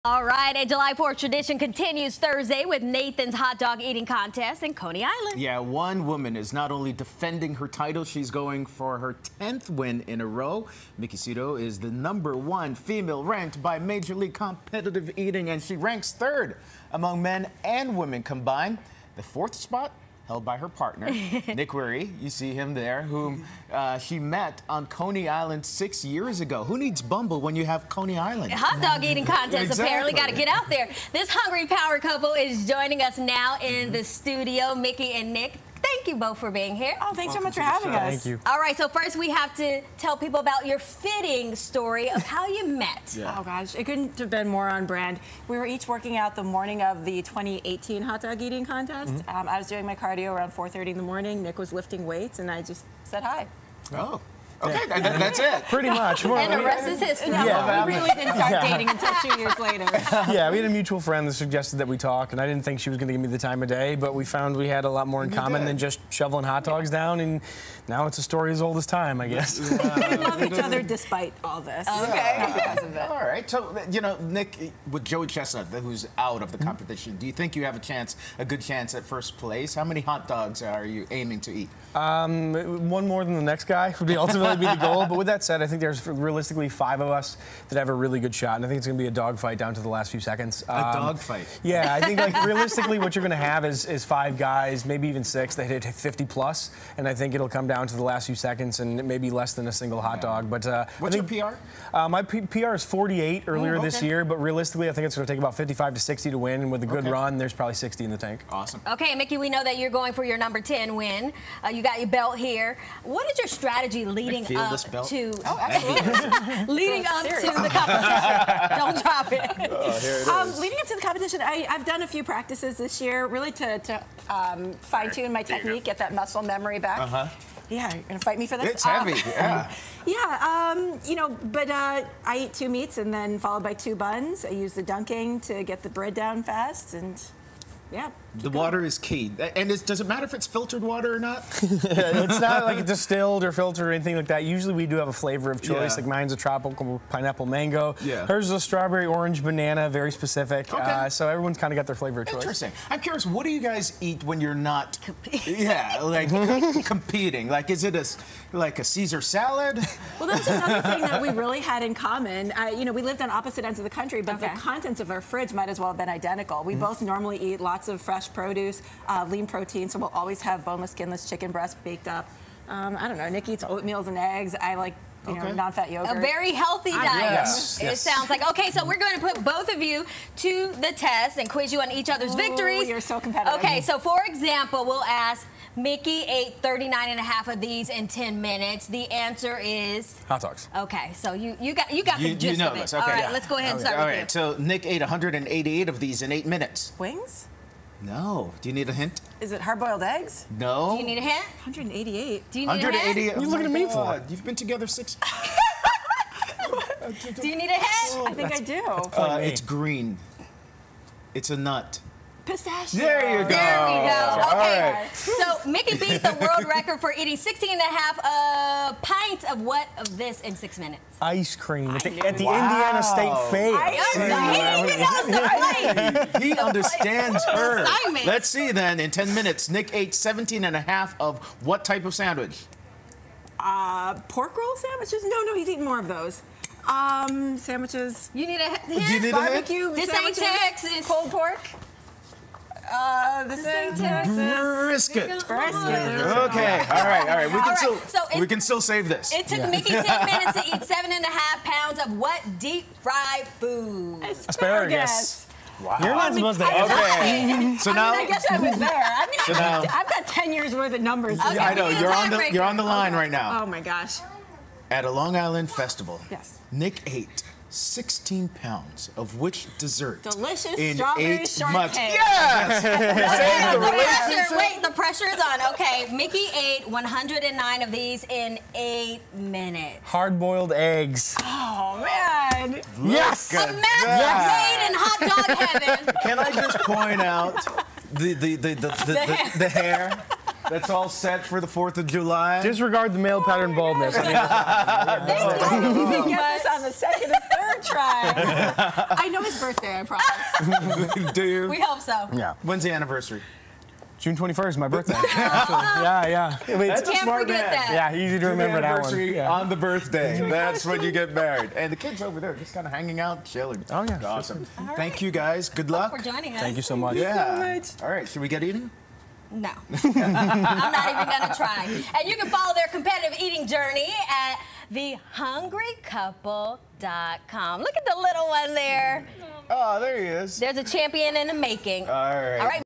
Couple meets at Nathan's hot dog eating contest in NYC
A Fourth of July tradition continues Thursday with Nathan's Hot Dog Eating Contest in Coney Island.